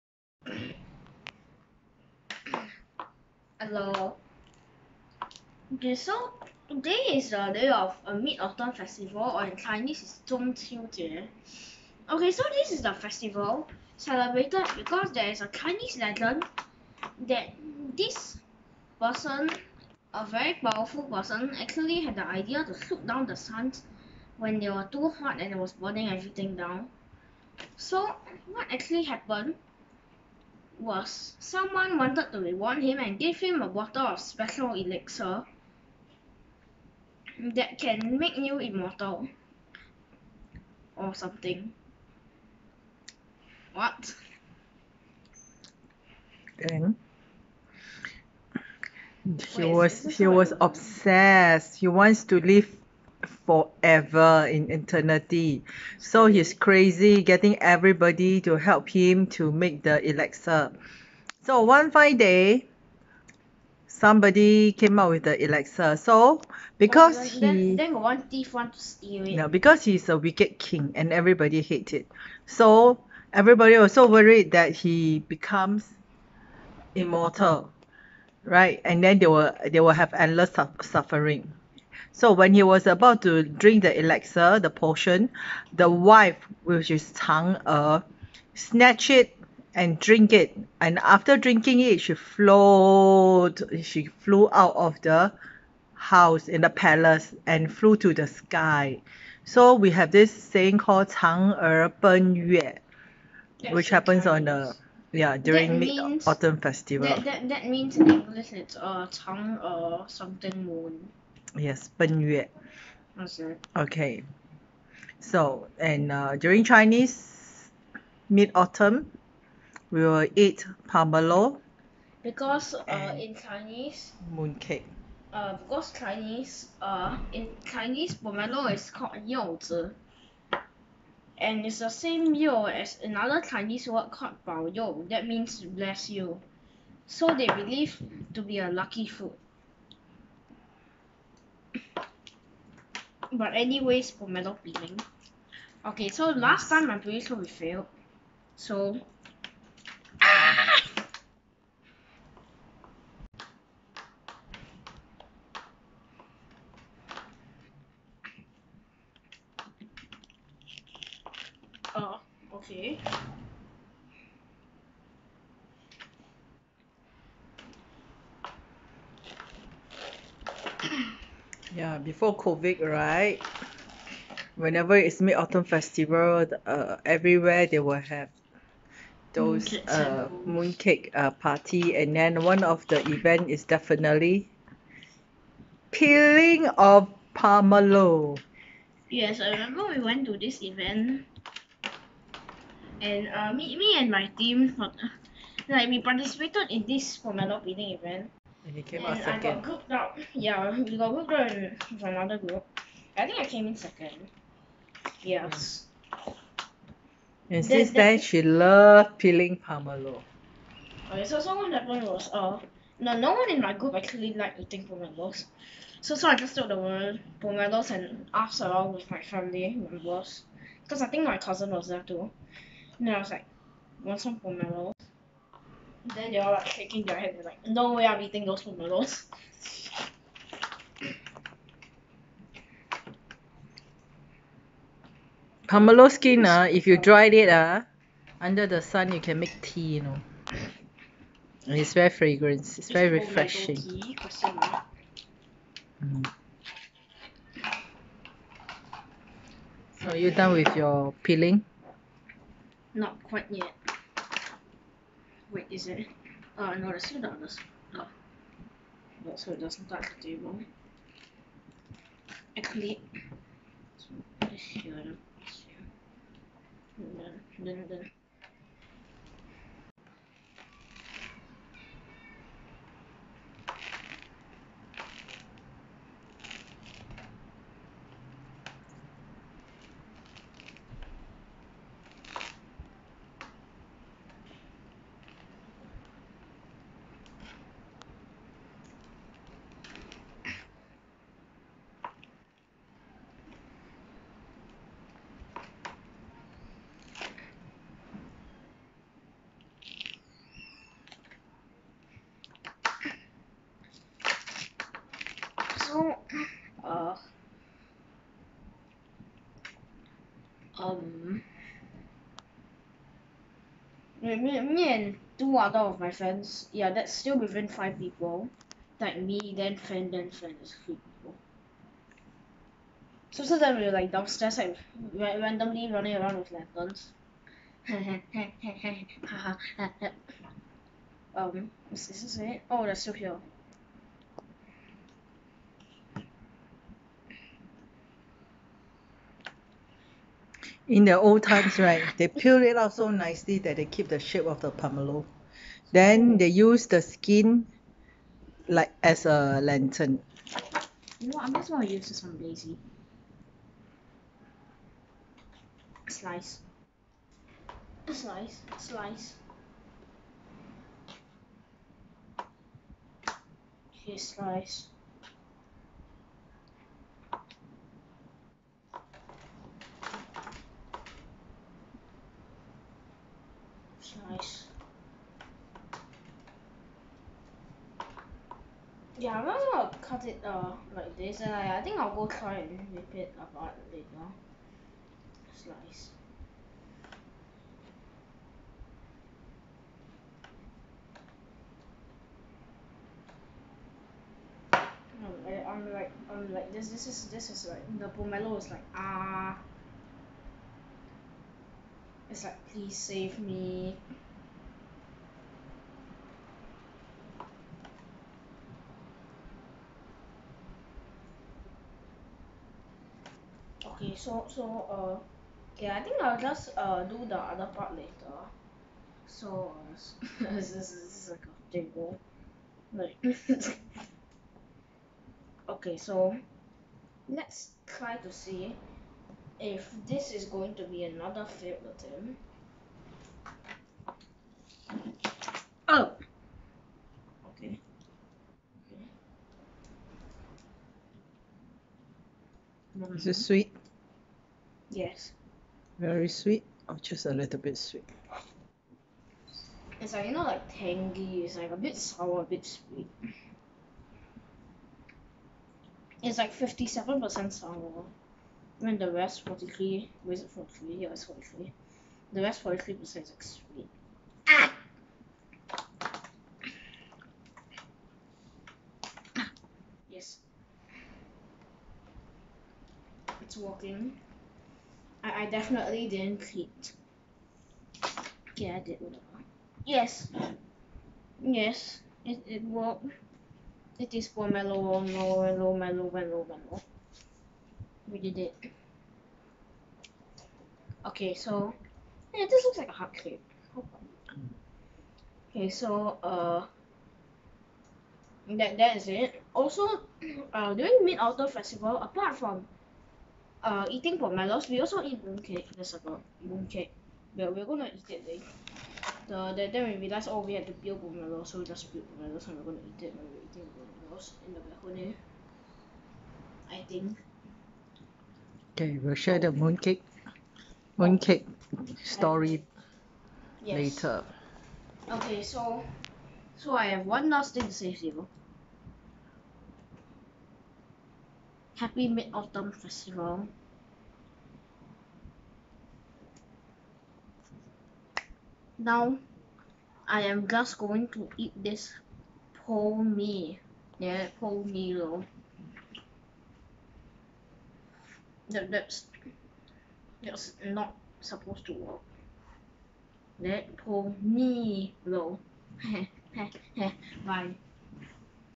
Pomelo peeling and why we peel them.
Sorry for late upload. This year's mid autumn is on 21 September.